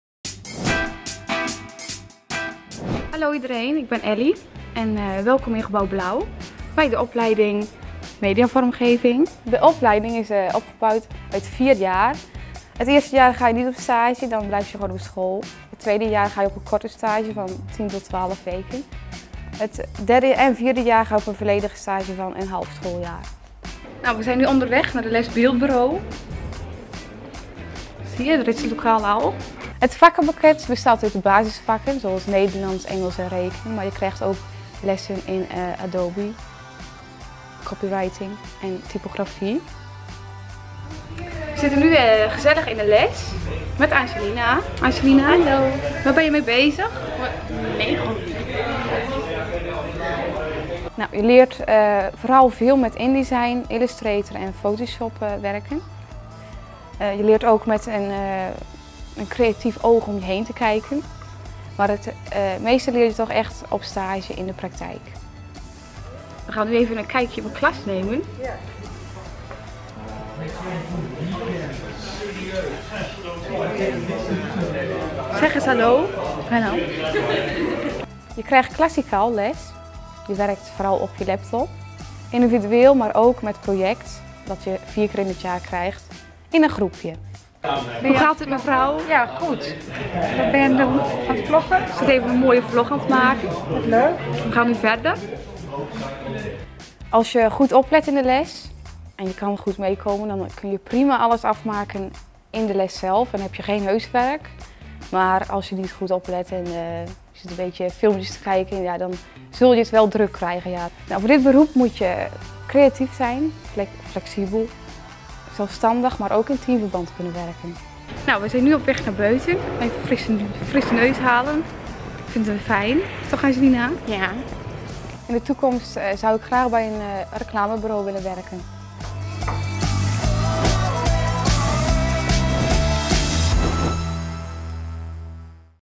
Mediavormgever: de opleiding
Elly volgt de opleiding Mediavormgever. Ze vertelt over wat ze leert op school: de theorievakken, de praktijklessen, de leraren en haar medestudenten. Met deze opleiding kan zij later aan het werk als Mediavormgever bij diverse bedrijven.

Zelf ervaren hoe het is om op Deltion te studeren? Meld je dan aan voor één van de meeloopdagen via de of kom naar de open dagen van Deltion

Al een studiekeuze gemaakt?